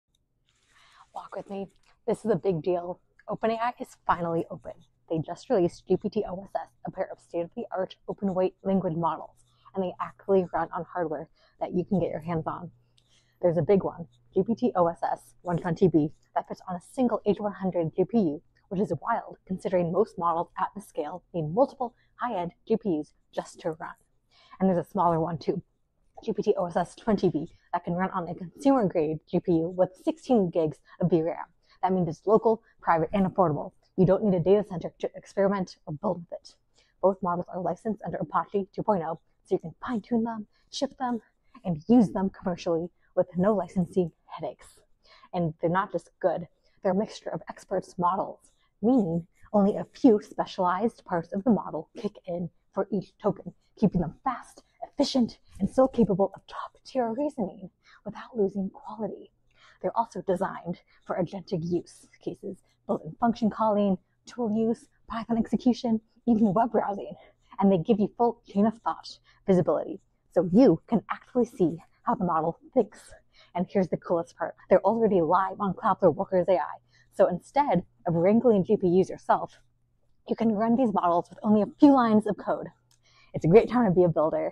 Walk with me--openai oss models!
OpenAI just launched their open source models! Walk with me and let's talk about them, why this is big news, and how you can run them on Cloudflare.